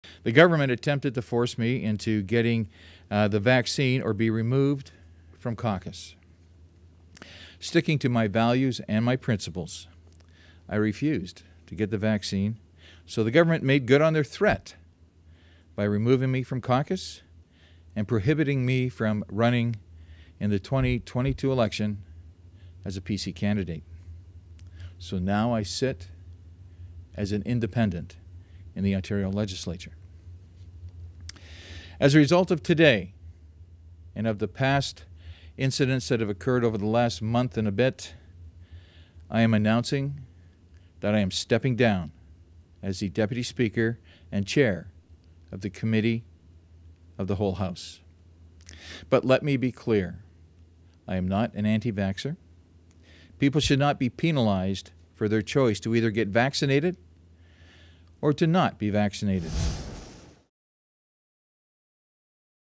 MPP ousted from PC Party caucus over vaccine status steps down as deputy speaker
Rick Nicholls, a former PC MPP who was ousted from cabinet for refusing to get the COVID-19 vaccine, announced Tuesday that he is stepping down from his role as deputy speaker.